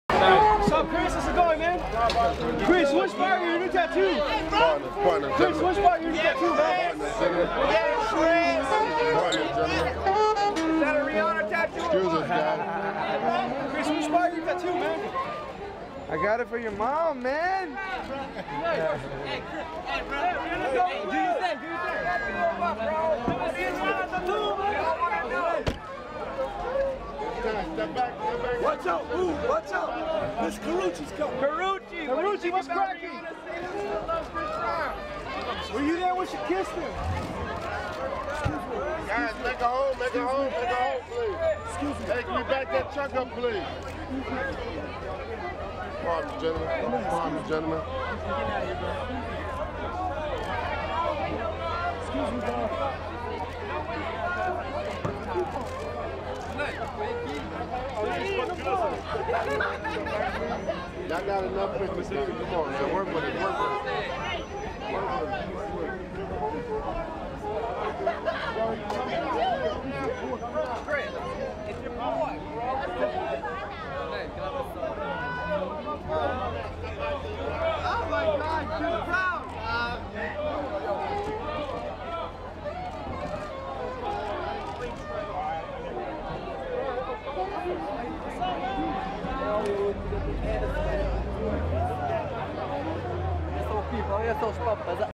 Chris Brown with Karrueche Tran depart Supperclub in Holl...
Chris Brown with Karrueche Tran depart Supperclub in Hollywood, 09/11/12